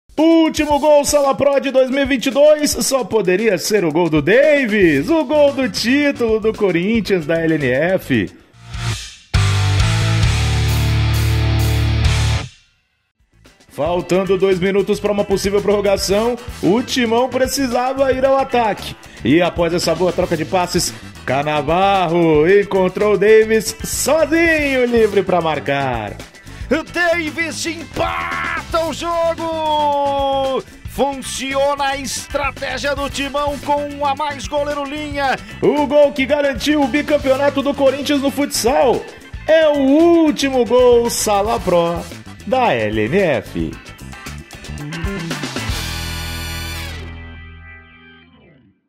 LNF 2022 - Gol das Finais - Volta
Deives, do Corinthians, marcou o gol do título e o mais bonito da volta das Finais da LNF 2022